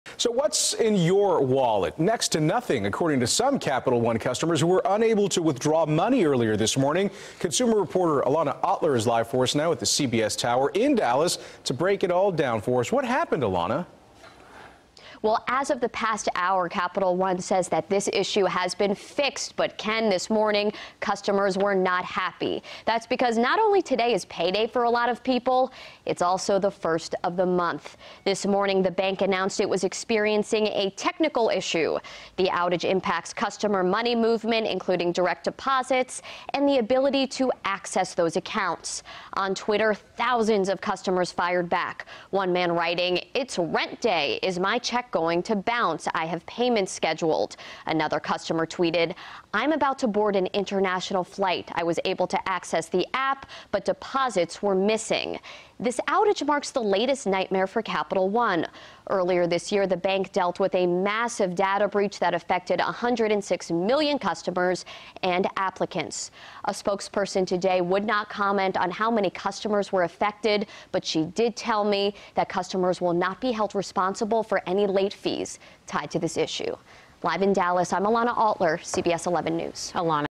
Capital One Customers Unable To Withdraw Money
Capital One customers were not happy Friday when they found they were unable to withdraw money from their bank accounts.